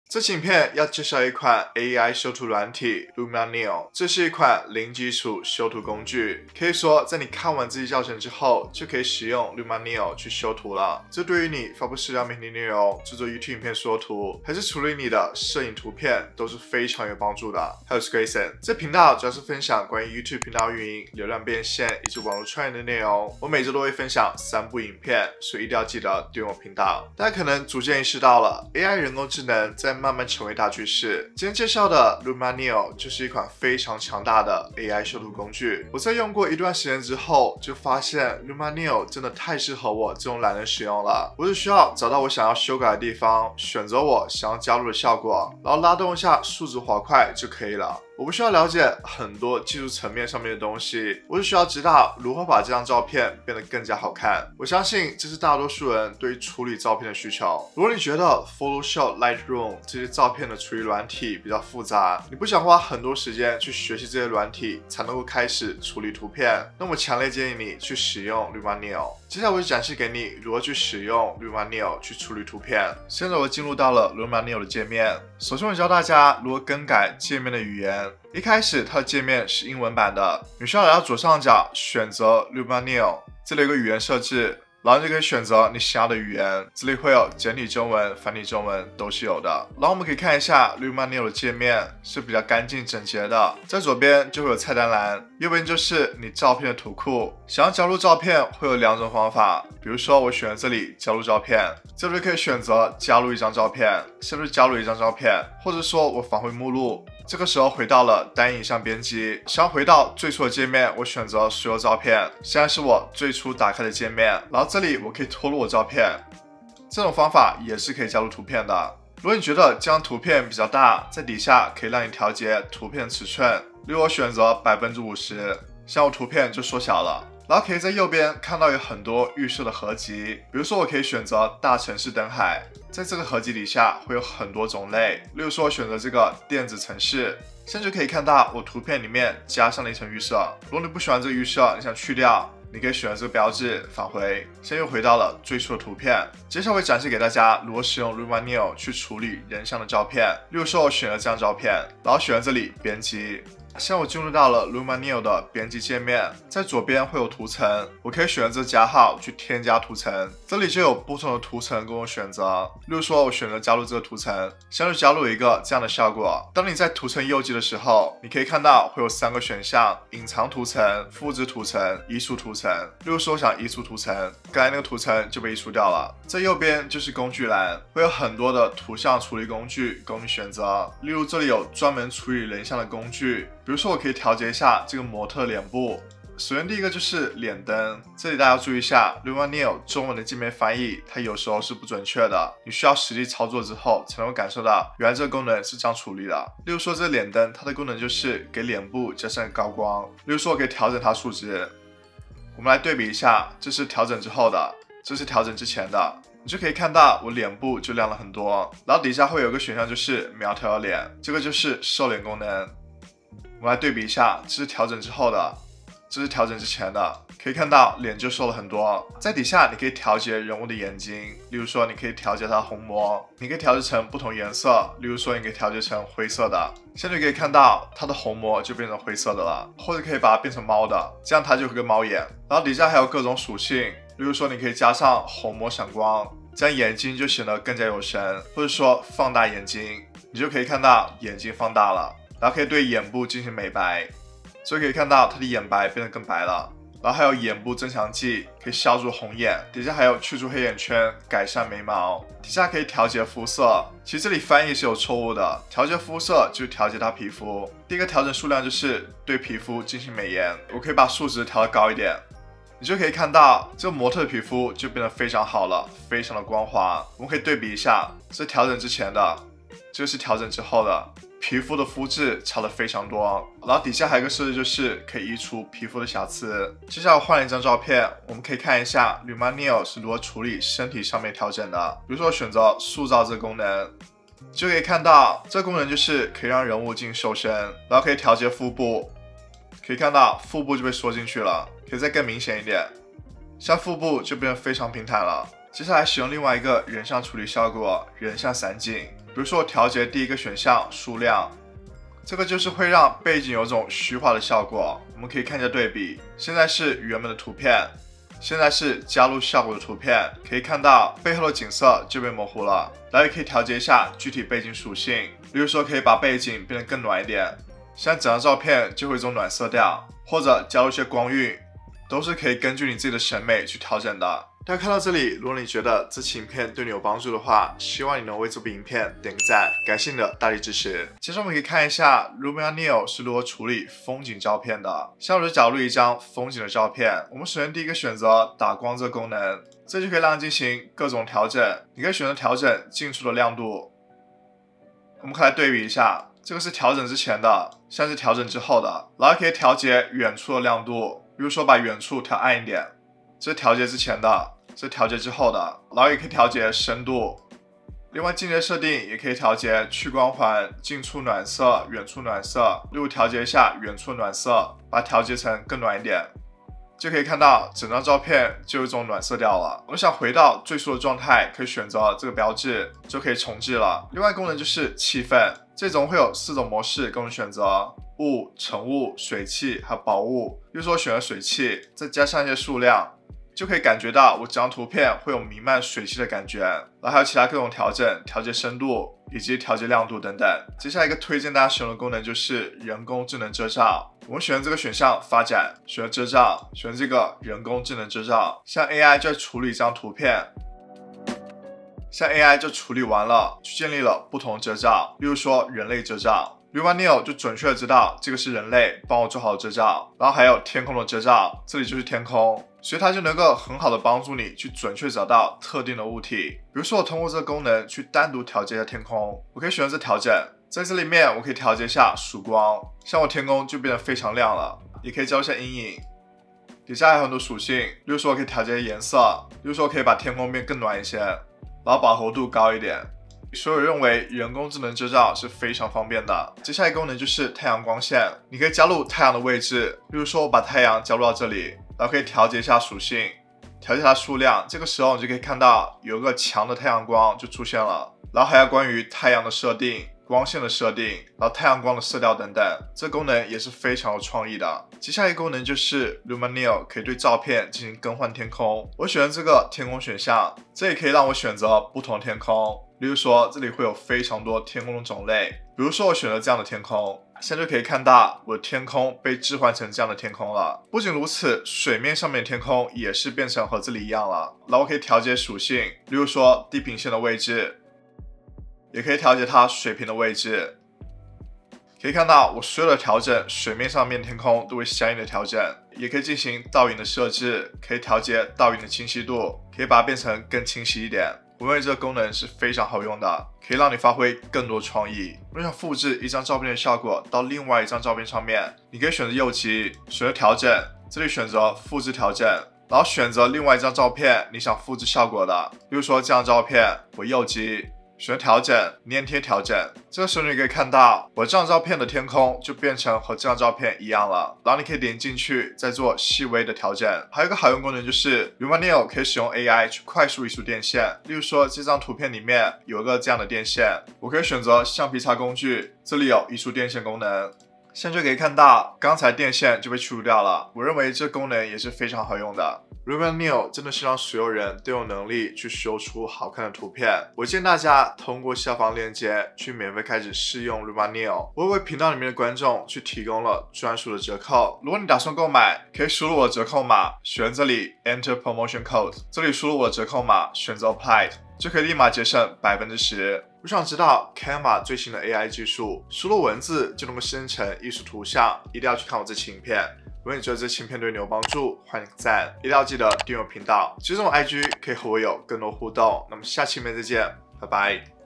【任何人用它都可以變成修圖大師】AI修圖軟體Luminar Neo
這期影片要介紹一款AI修圖軟體Luminar NEO, 這是一款零基礎修圖工具。可以說當你看完這期影片之後，你就能夠開始用Luminar NEO去修圖了。這對於你發社交媒體內容，製作影片縮圖，還是處理你的攝影圖片都是非常有幫助的。

✦ 最全的YouTube無版權素材模板庫 Envato Elements (7天免費試用) ‣‣

時間軸：
0:00 介紹
0:27 最適合零基礎新人的修圖工具
1:09 界面介紹
2:28 人像處理
5:31 風景處理
8:53 折扣碼使用